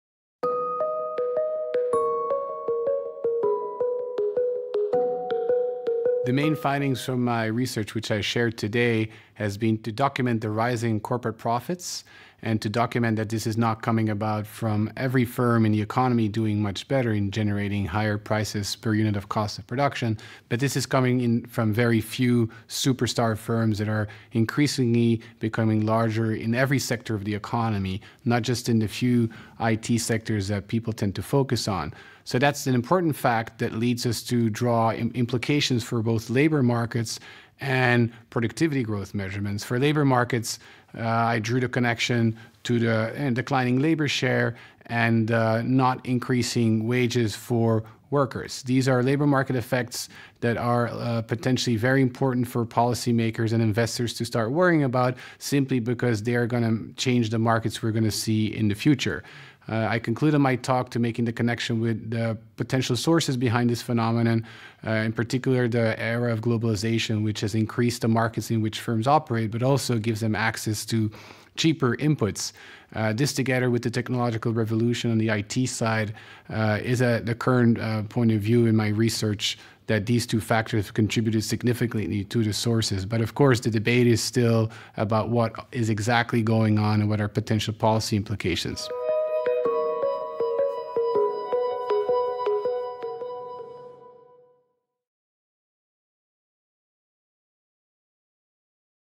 BNPPAMIF18 | Interview with Jan de Loecker
Interview with Jan de Loecker, Professor of Economics, KU leuven, Visiting Professor, Princeton, during the BNP Paribas Asset Management Investment Forum 2018.